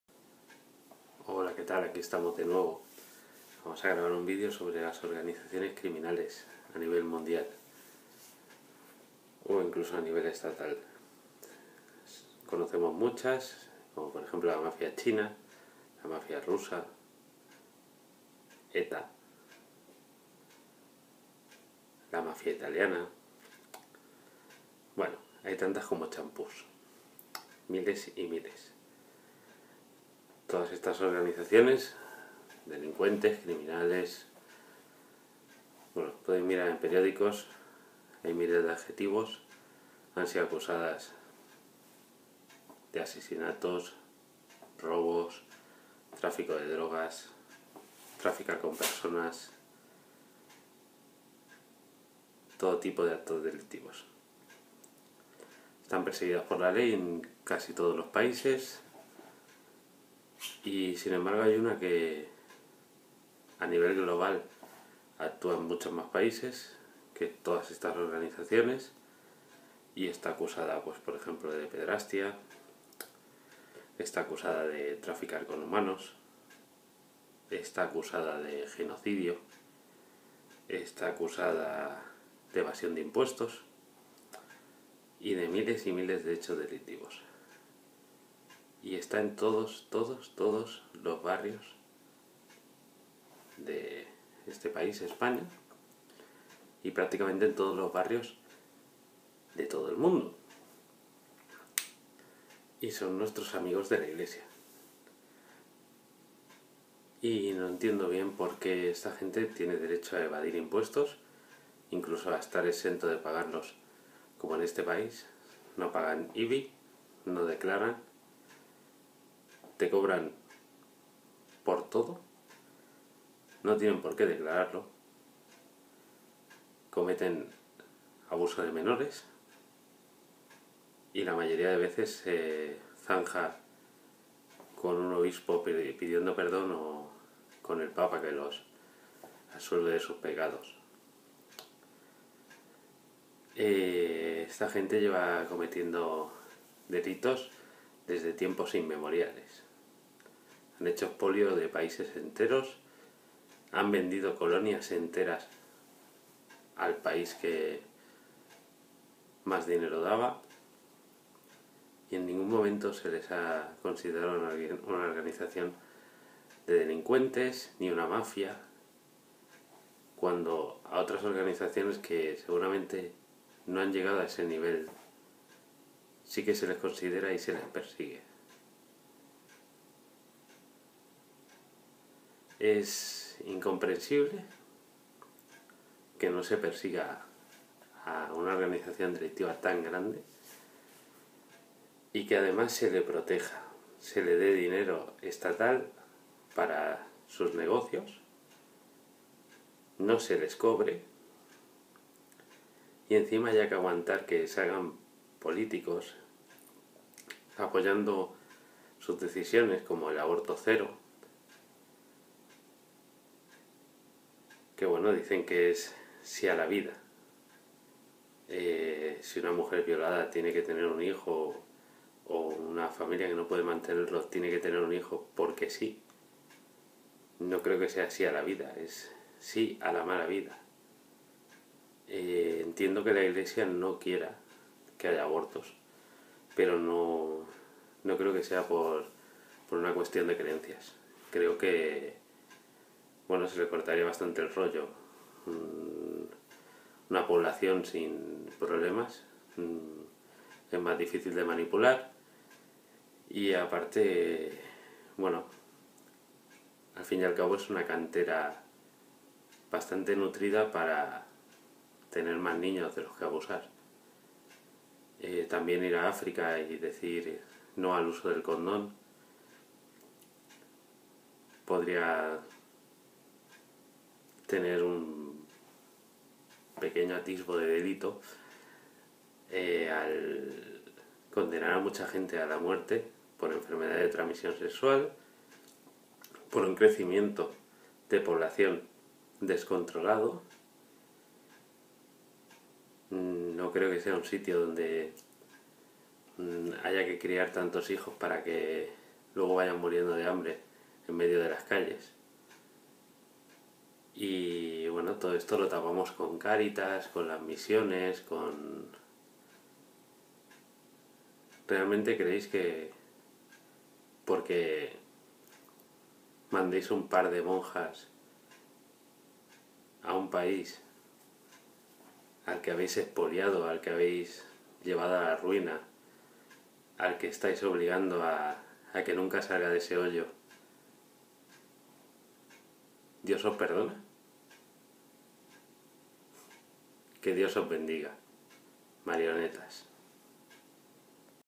La peor mafia tiene sede en tu barrio
La peor organización criminal de todos los tiempos y nadie la persigue. Y eso?